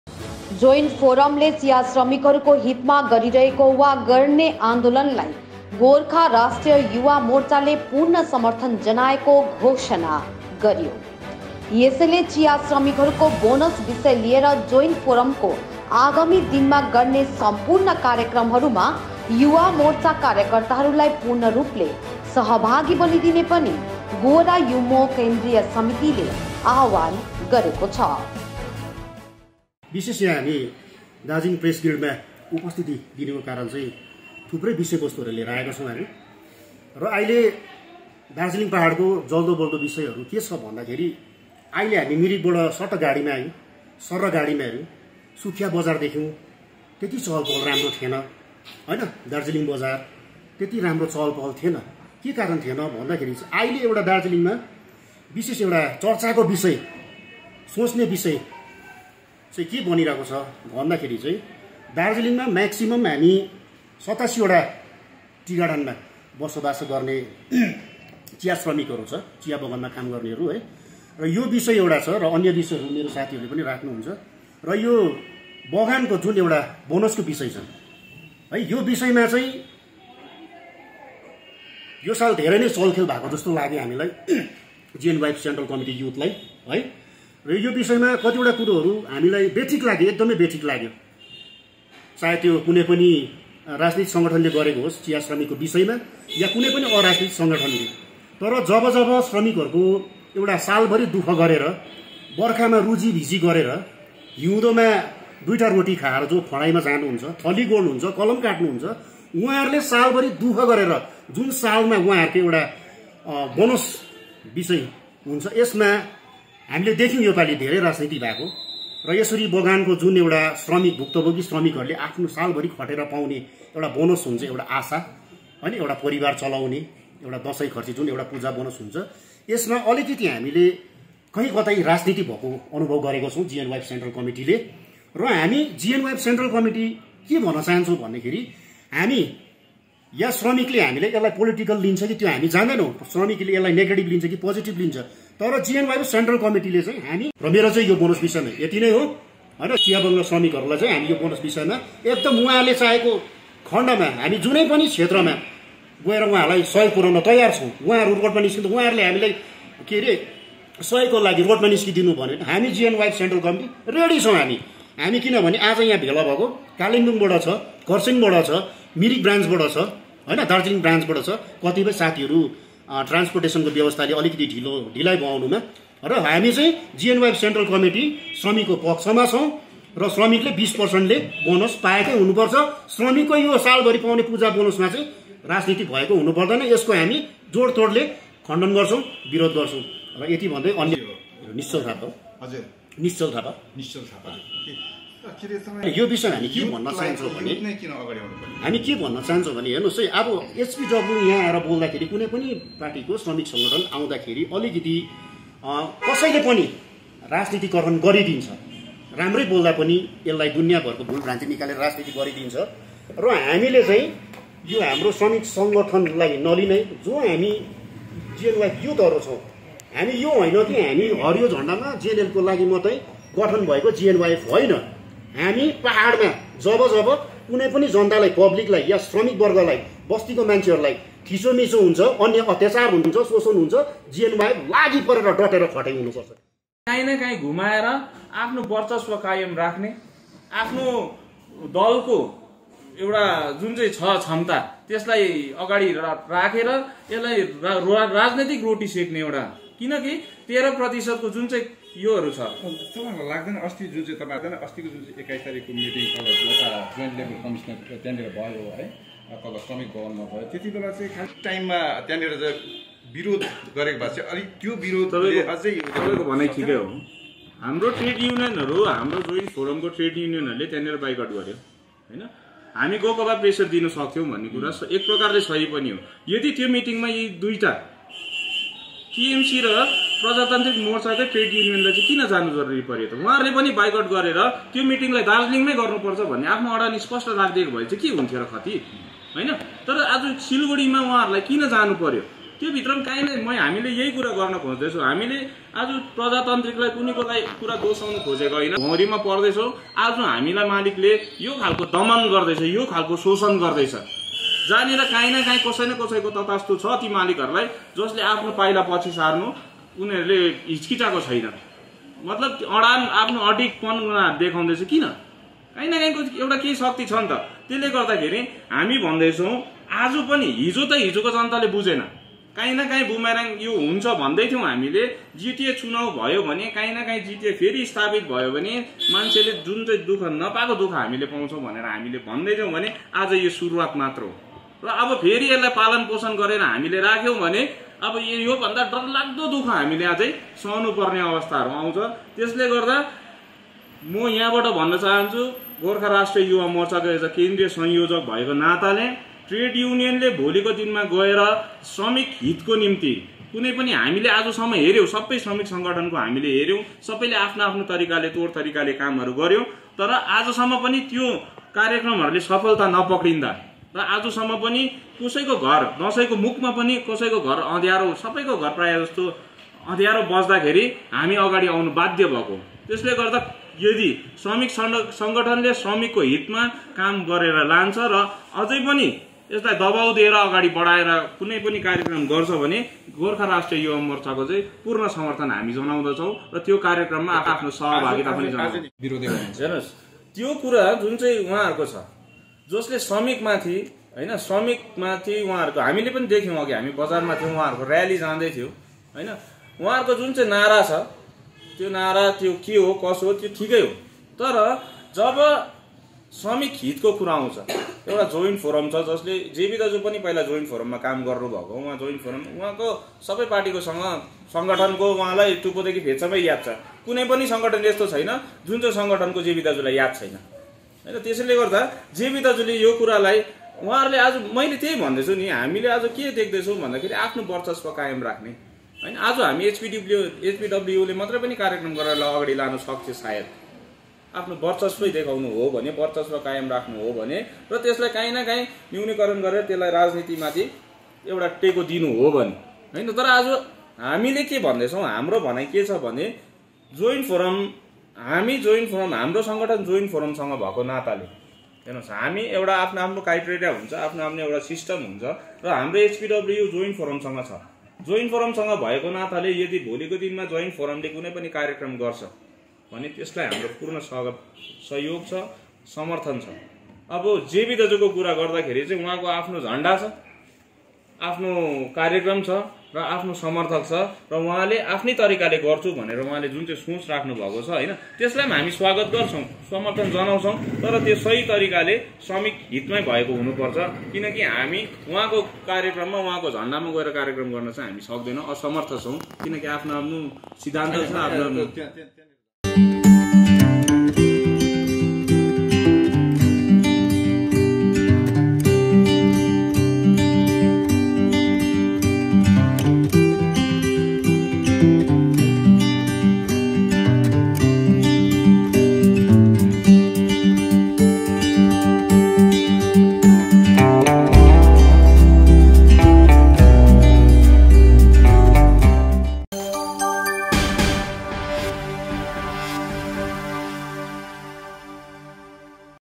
GNLF youths back Joint Forum tea bonus stir
जोइण्ट फोरमले चियाश्रमिकहरूको हितमा गरिरहेको आन्दोलनलाई गोर्खा राष्ट्रिय युवा मोर्चाको पूर्ण समर्थन